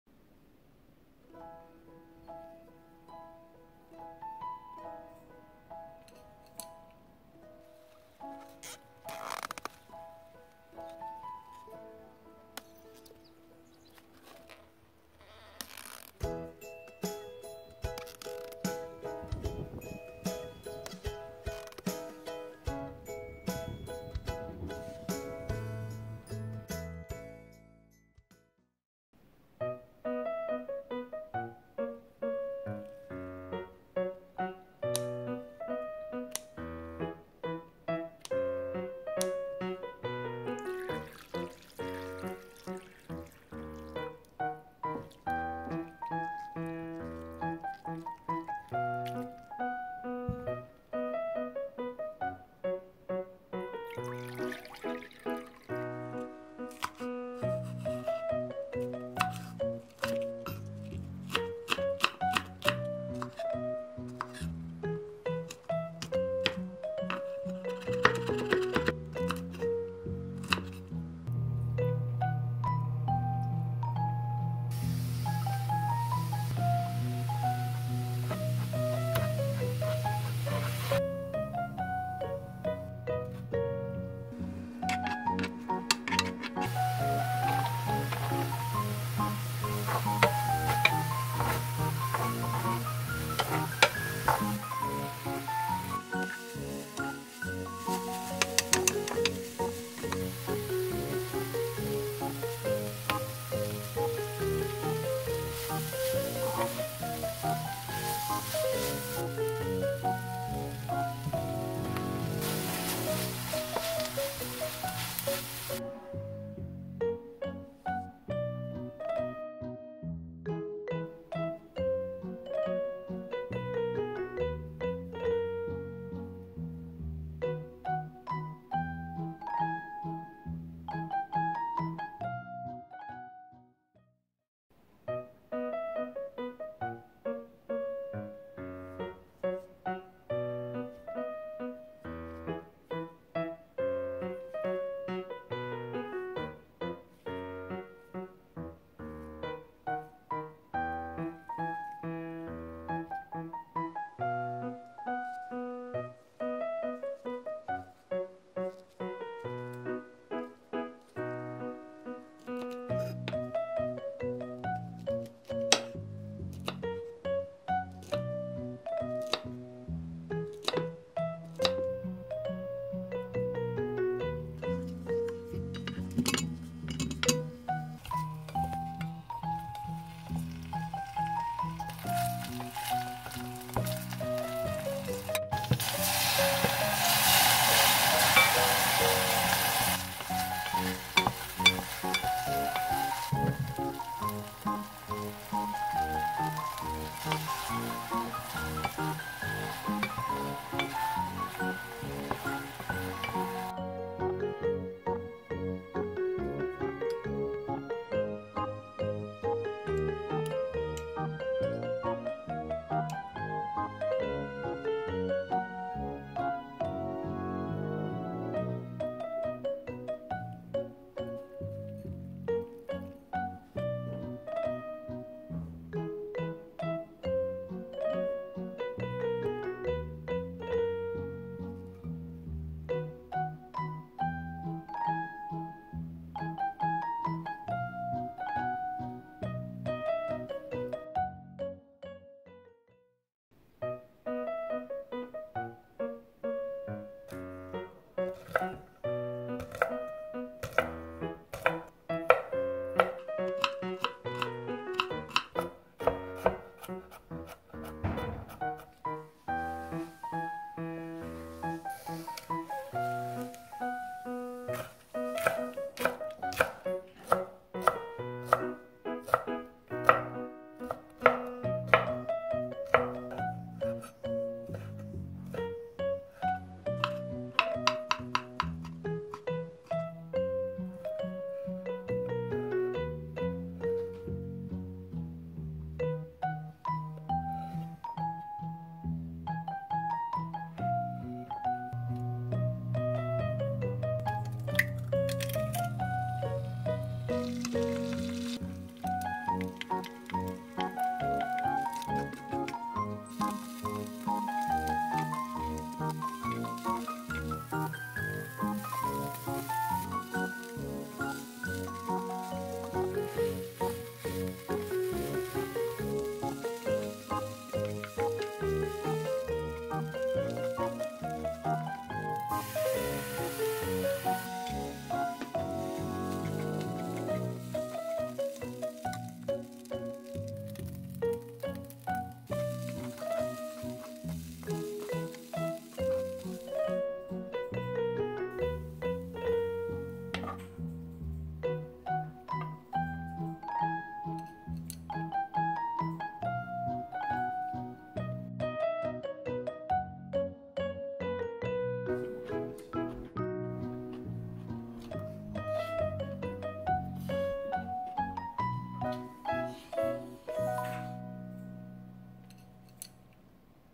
텃밭에서 수확한 마늘종요리ㅣ진미채마늘종볶음ㅣ마늘종어묵조림ㅣ마늘종볶음밥
안녕하세요. 여리입니다.
작년 늦가을에 텃밭에 심었던 마늘이 벌써 마늘종이 생겼어요.
100쪽정도 심어서 조금이긴 하지만 귀하게 야무지게 먹어봅니다.
이제 마늘종도 뽑아주었으니 땅속의 마늘이 쑥쑥 크겠죠? 6월말이 기대됩니다.^^
즐거운 텃밭생활하세요.